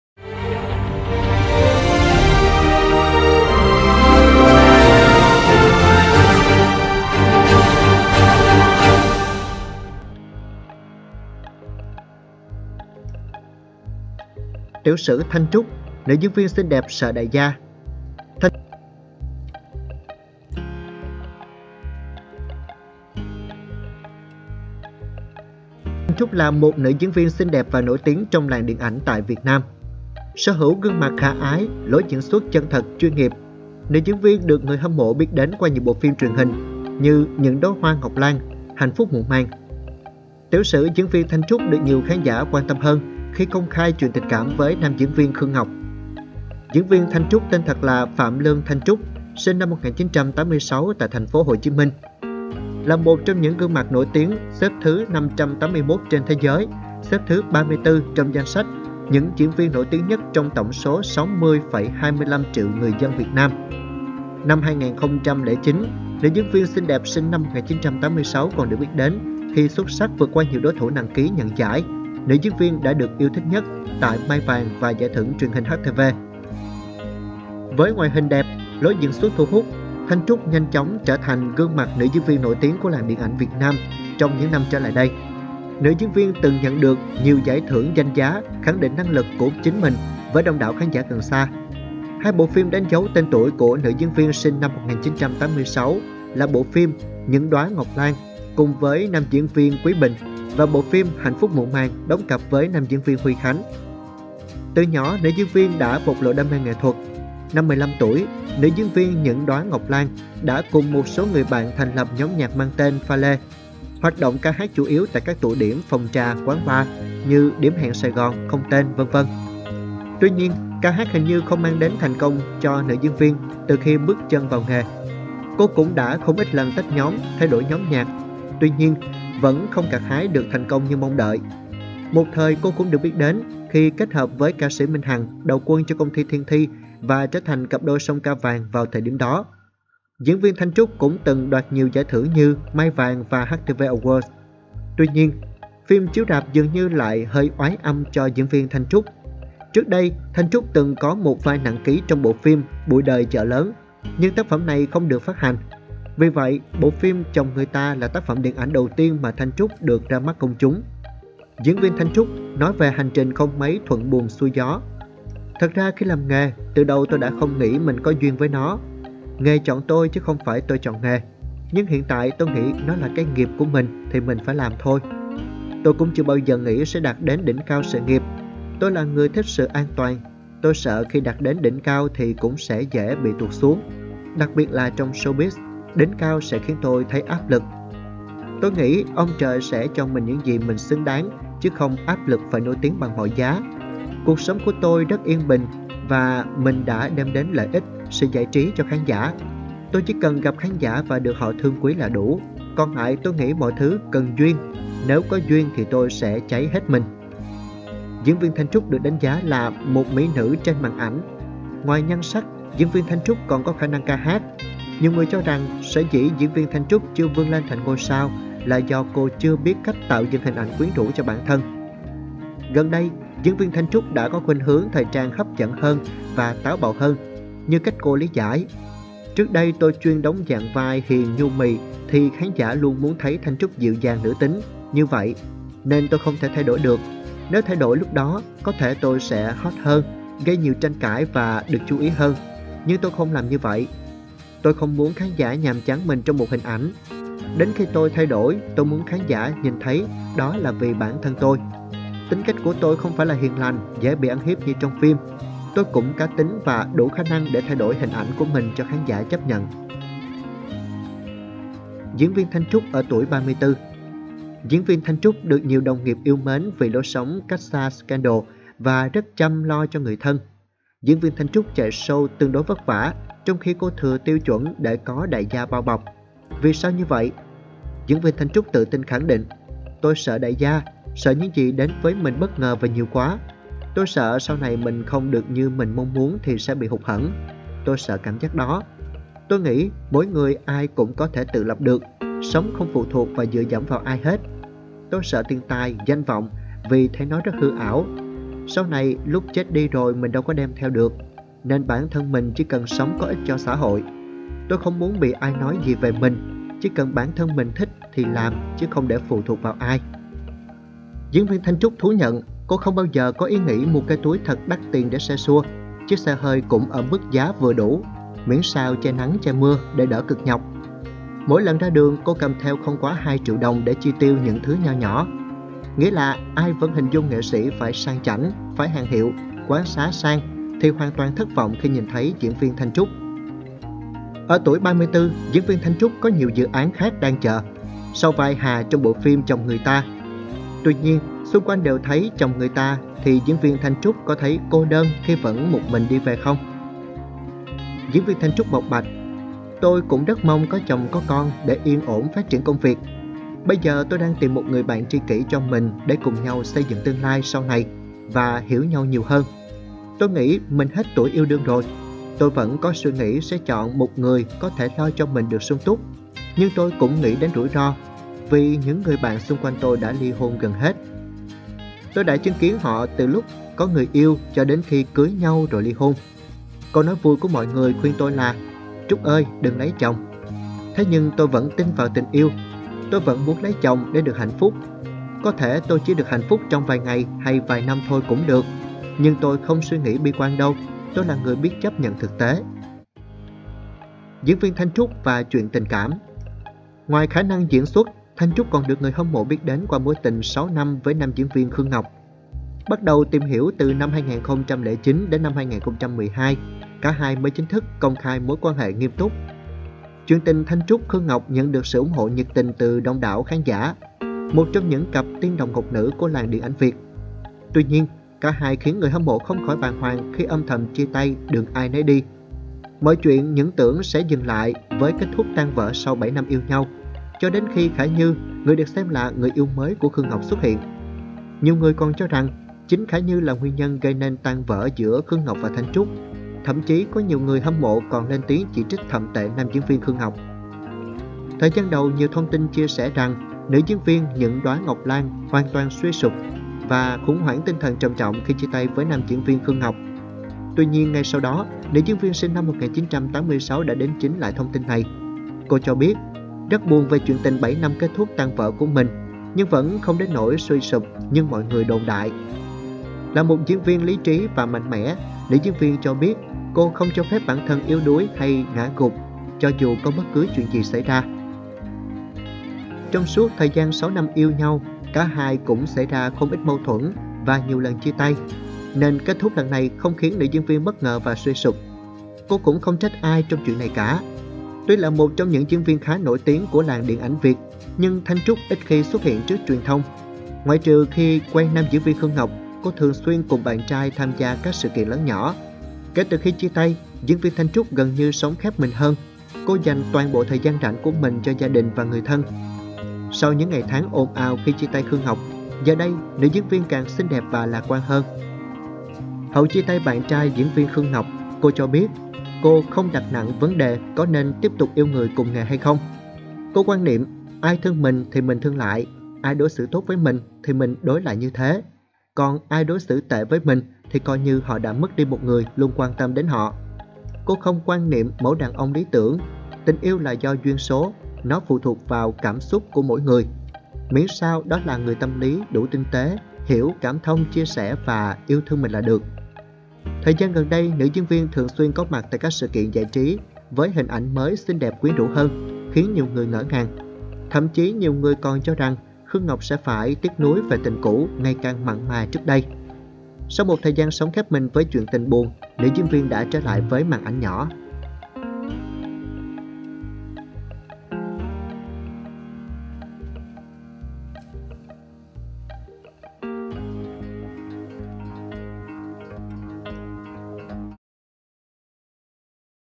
Tiểu sử diễn viên THANH TRÚC Người đẹp lận đận của màn ảnh và không có duyên với đại gia
Phạm Lương Thanh Trúc còn được biết đến với nghệ danh Thanh Trúc, là một diễn viên trẻ người Việt Nam, ngày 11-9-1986 tại Thành phố Hồ Chí Minh. Với ngoại hình đẹp, lối diễn xuất có triển vọng Thanh Trúc đang là một cái tên nổi lên trong giới điện ảnh Việt Nam.
Cô đã từng giành được giải thưởng "Nữ diễn viên được yêu thích nhất" tại lễ trao giải thưởng Mai Vàng và tại Giải thưởng Truyền hình HTV năm 2009. Đối với khán giả màn ảnh nhỏ, họ ấn tượng với Thanh Trúc qua vai diễn trong bộ phim "Những đóa ngọc lan", diễn chung cùng nam diễn viên Quý Bình và bộ phim "Hạnh Phúc muộn màng" của nam diễn viên Huy Khánh.

Năm 15 tuổi, Trúc đã lập ra nhóm Pha Lê và đi hát ở phòng trà, quán bar như Không tên, Điểm hẹn Sài Gòn. Nhưng dường như cô không có duyên với nghiệp hát nên loay hoay mãi mà chẳng gặt hái được gì, hết tách nhóm Pha Lê rồi lập nhóm GPI. Thanh Trúc và Minh Hằng cũng từng là cặp đôi song ca mang tên J8687 do công ty Thiên Thi quản lý hơn 7, 8 năm trước.